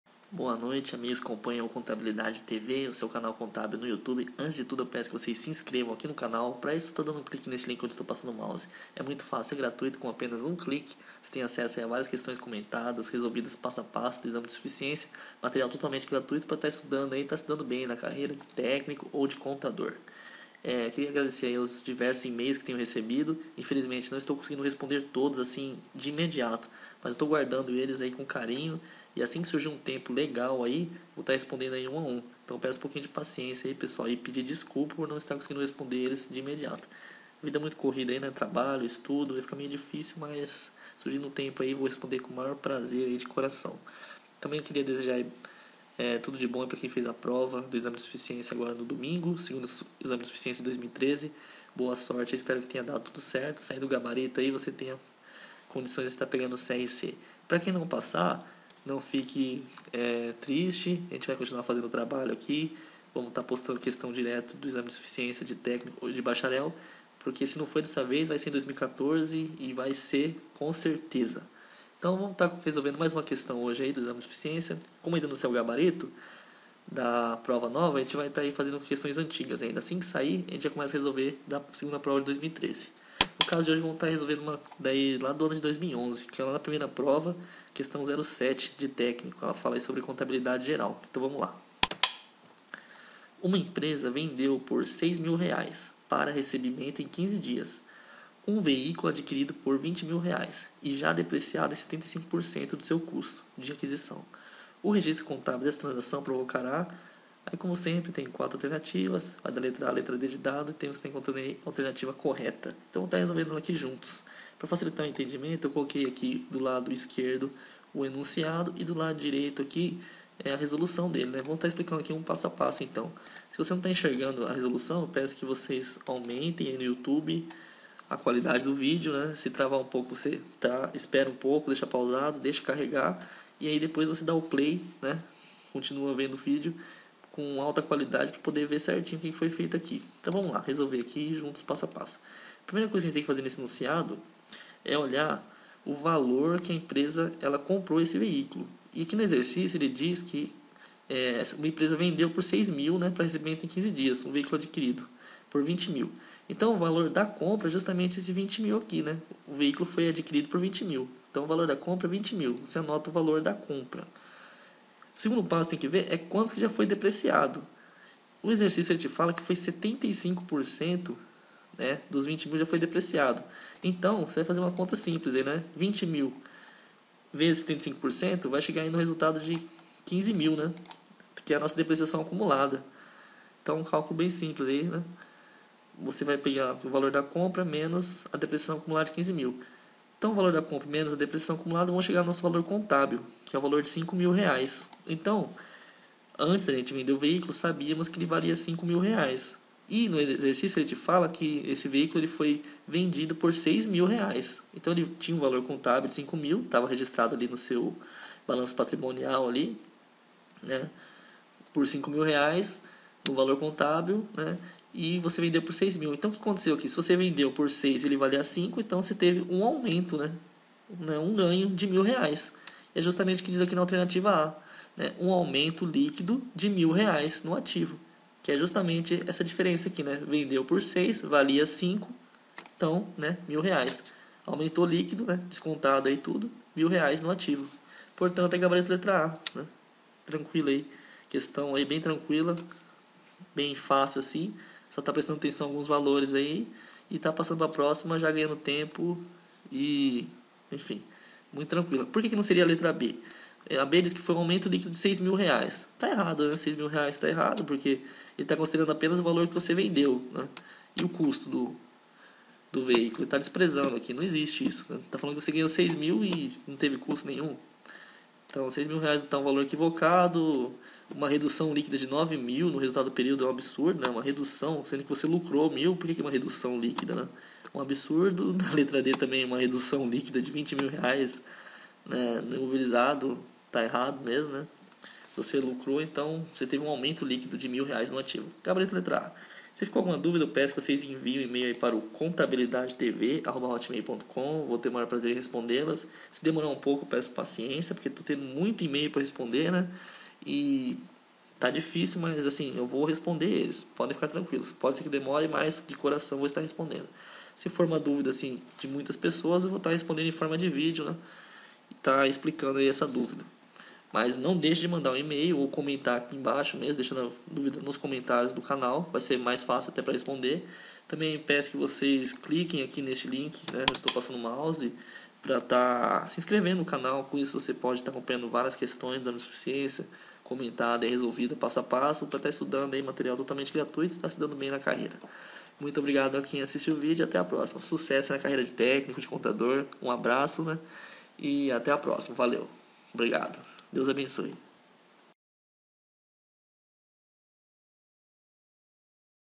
EXAME DE SUFICIÊNCIA Técnico em Contabilidade 2011 prova 1 Questão 07 Contabilidade Geral
7. Uma empresa vendeu por R$6.000,00, para recebimento em 15 dias, um
veículo adquirido por R$20.000,00 e já depreciado em 75% do seu custo de
aquisição. O registro contábil dessa transação provocará:
a) um aumento líquido de R$1.000,00 no Ativo.
b) um aumento líquido de R$6.000,00 no resultado do período.
c) uma redução líquida de R$9.000,00 no resultado do período.
d) uma redução líquida de R$20.000,00 no Imobilizado.

gabarito A